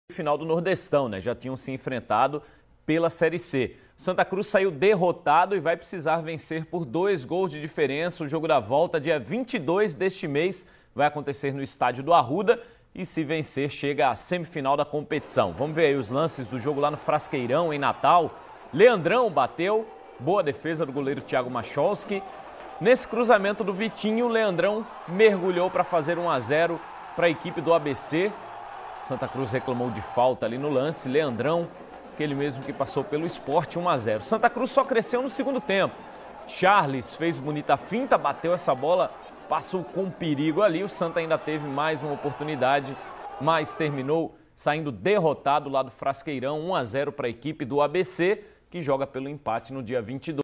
ABC 1 x 0 Santa Cruz - Copa do Nordeste 2018
01/05/2018
Copa do Nordeste - Quartas-Final
ABC 1 x 0 Santa Cruz
Gols: Leandrão 37 do 1º